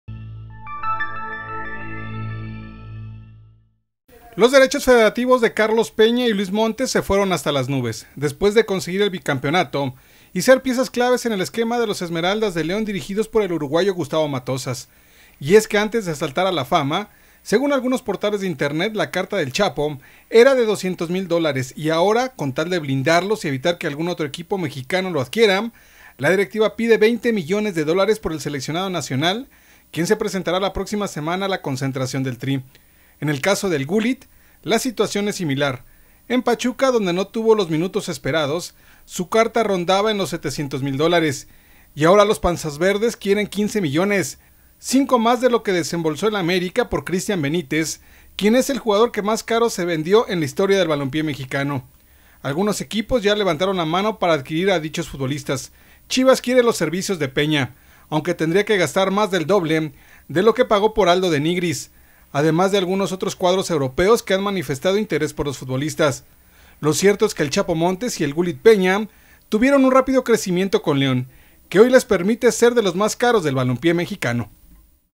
Se cotizan hasta las nubes Montes y Peña
CLAUSURA 2014, México D.F. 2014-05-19. Los derechos federativos de Carlos Peña y Luis Montes se fueron hasta las nubes, después de conseguir el bicampeonato y ser piezas claves en el esquema de los Esmeraldas de León dirigidos por el uruguayo Matosas.